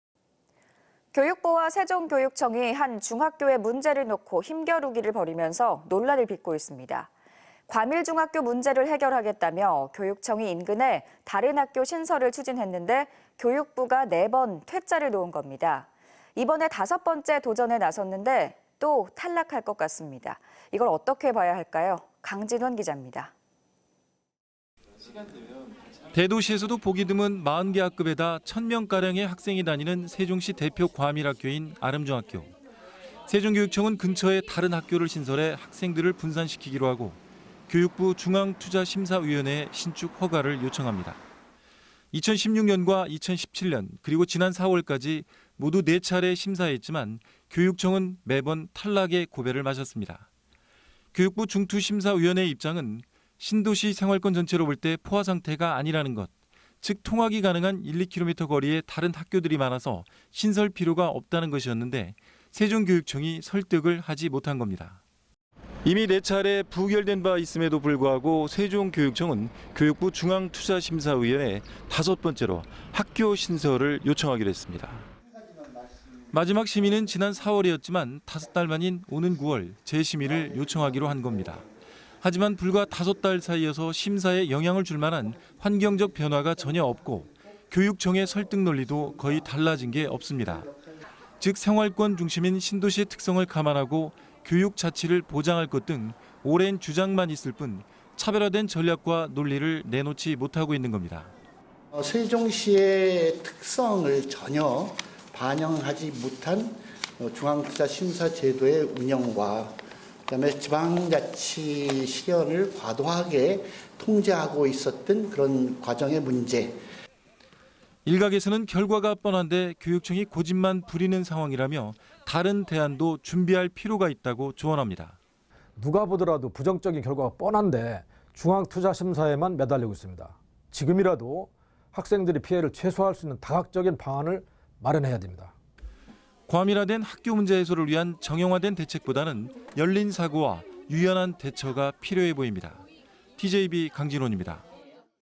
[TJB뉴스] 교육부는 안된다는데.. 다섯 번째 도전?
교육부와 세종교육청이
한 중학교의 문제를 놓고
힘겨루기를 벌이면서
논란을 빚고 있습니다.

과밀 중학교 문제를
해결하겠다며,
교육청이 인근에
다른 학교 신설을
추진했는데,
교육부가 4번
퇴짜를 놓은 겁니다.

이번에 다섯번째 도전에
나섰는데,
또 탈락할 것 같습니다.

이걸 어떻게 봐야 할까요?

강진원 기잡니다.